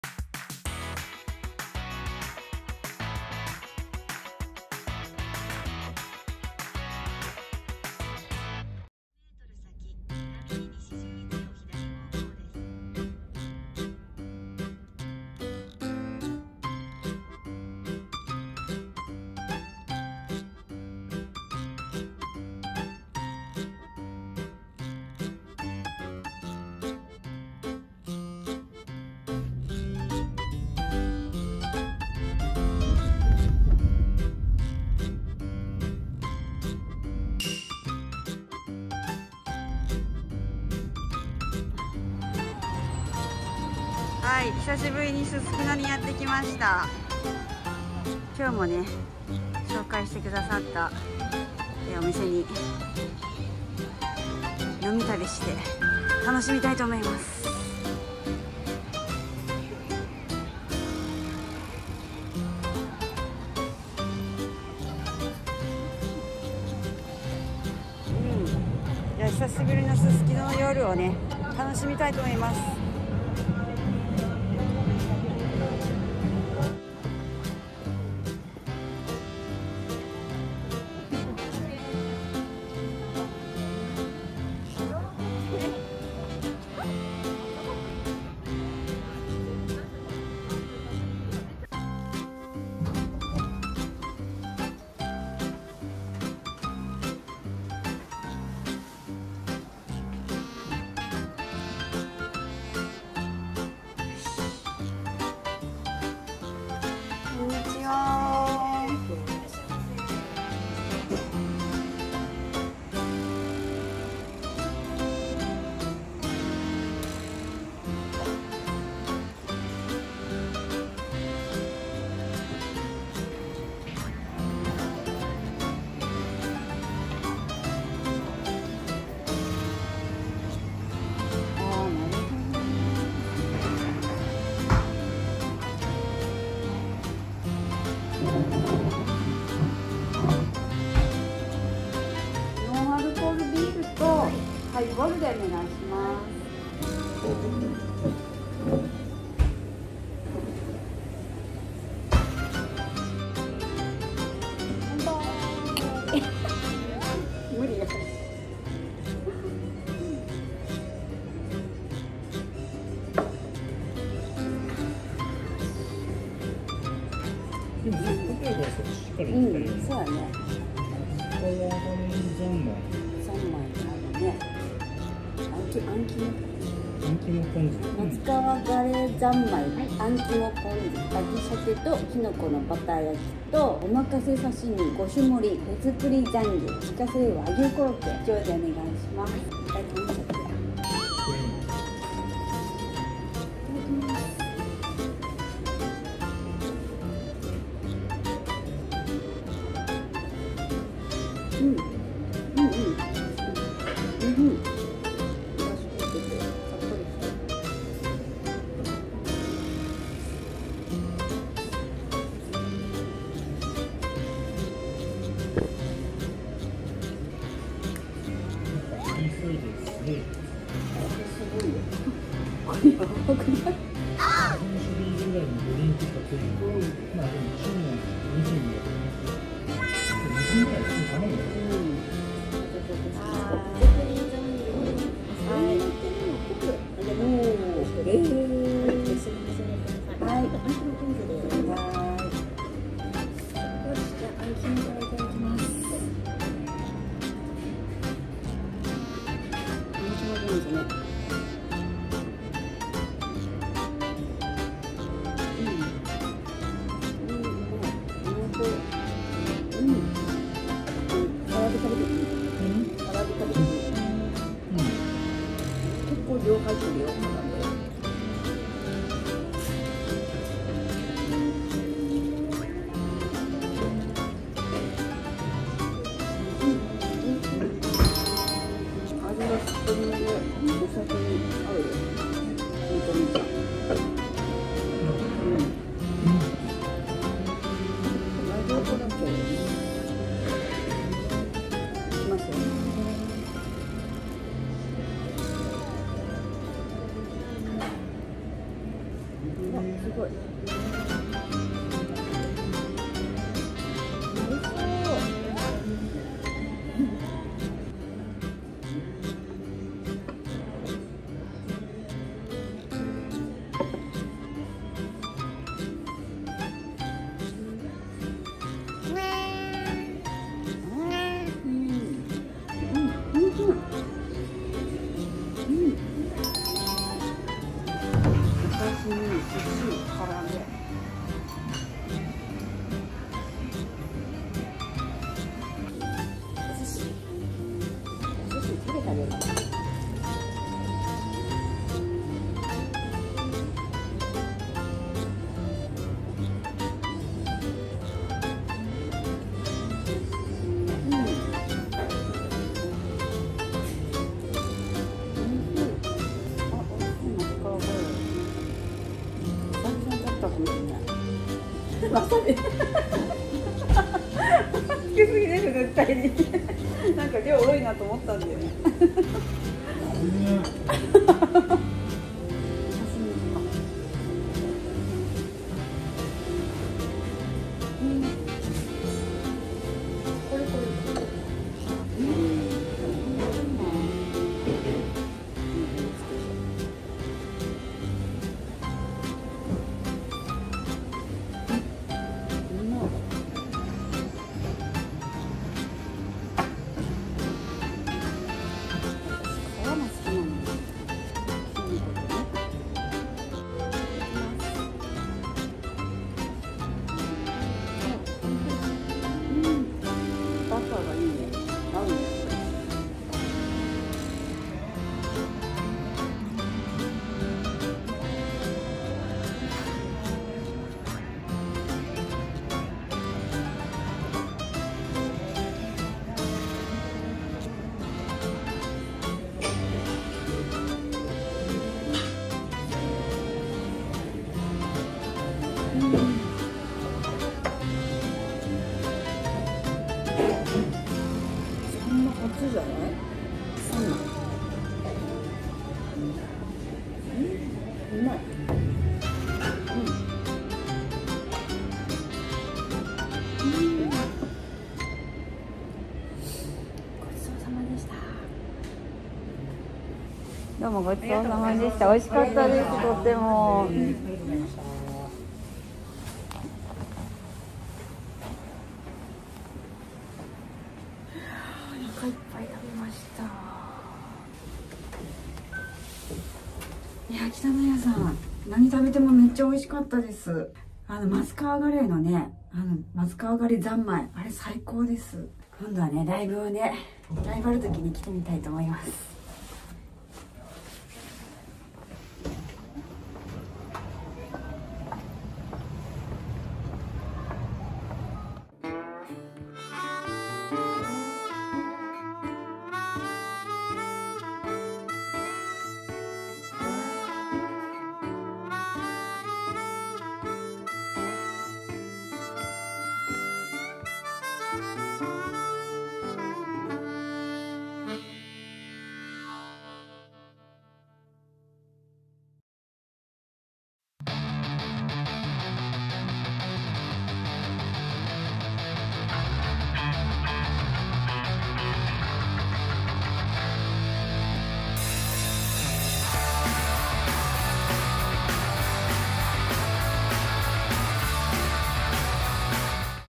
【北海道・札幌市】すすきので北の味覚を大満喫！「北乃家」さんのやさしいお料理で心も体もあったまる«٩(*´∀`*)۶»
今日はすすきのにあります「北乃家」さんへ(⁎˃ᴗ˂⁎)
「知床小さな居酒屋まりの店」のまりママからご紹介頂きました！ありがとうございます！
なんとこちらでは不定期に店内ライブも開催されているとのこと💕
お料理も旬の食材を使われており、まさに「ライブ感」がありました٩(๑˃̵ᴗ˂̵)و
今回は松川カレイやあん肝が旬でしたが、いつの時期に行ってもその時期の旬が味わえる素敵なお店です❤️

エリlogチャンネル SNSやっております( ᴗ͈ˬᴗ͈)

撮影は  iPhone13、iPhone11を使用しています。
編集ソフトはFCPX、PCは14インチMacBook Proです。